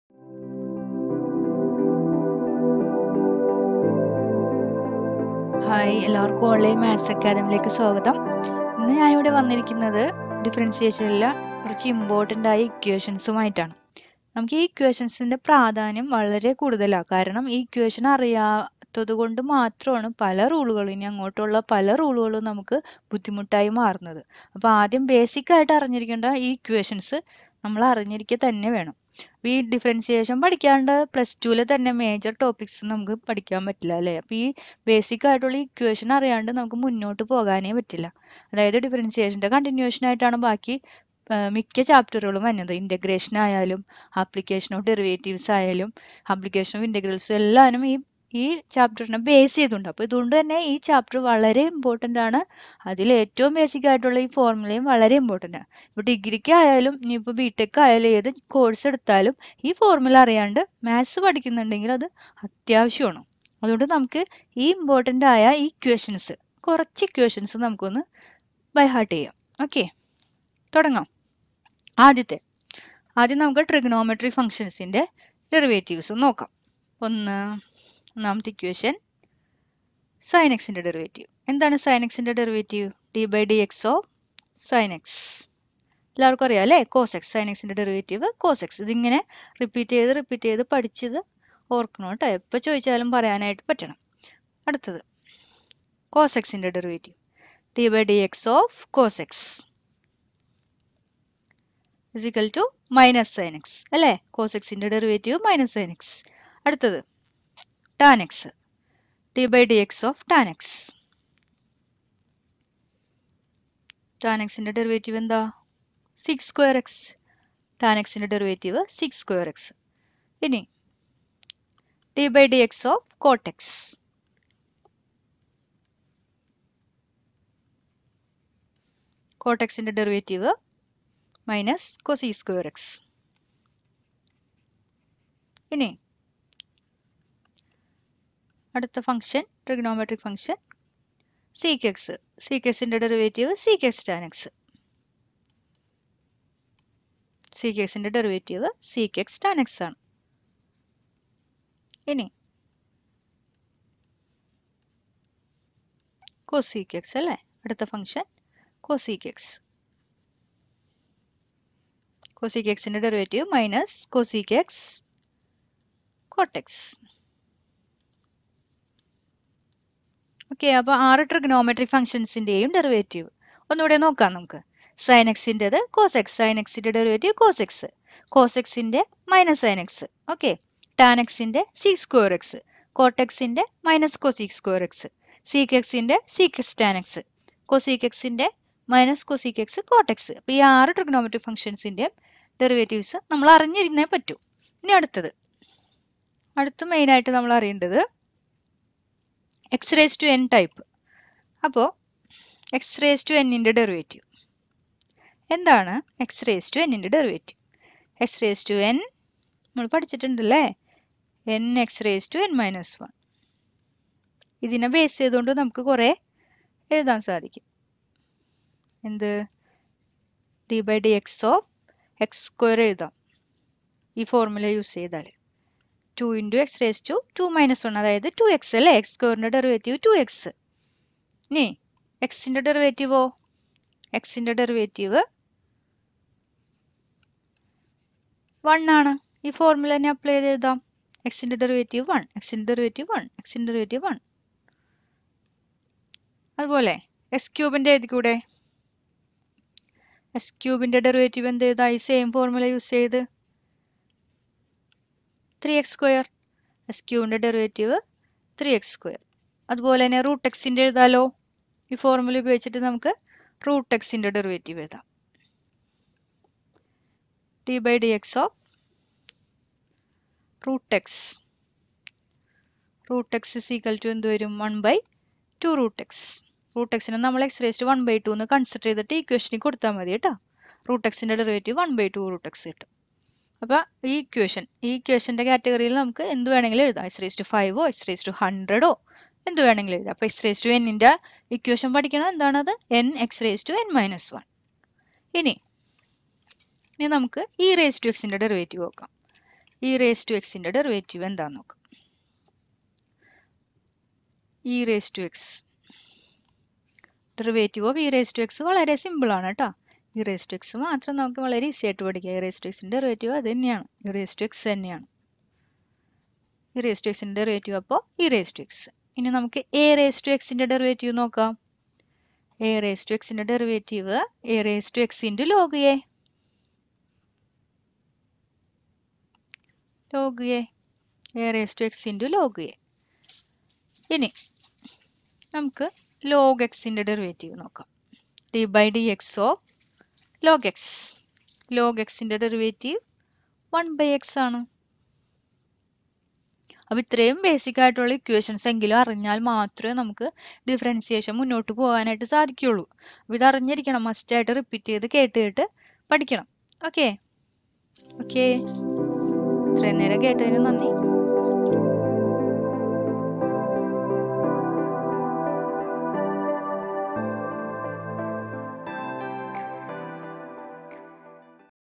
Maths Tuition: Differentiation - 5 മിനുറ്റിൽ Differentiation Formulae പഠിക്കാം in Malayalam
5 മിനുറ്റിൽ Differential ഫോർമുലാസ് പഠിക്കാം... +1, +2  കുട്ടികൾക്ക് വളരെ ഉപകാരപ്രദമാകുന്ന ഒരു കോഴ്സ്. ഇഷ്ടപ്പെട്ടെങ്കിൽ ലൈക് ചെയ്യാൻ മറക്കല്ലേ..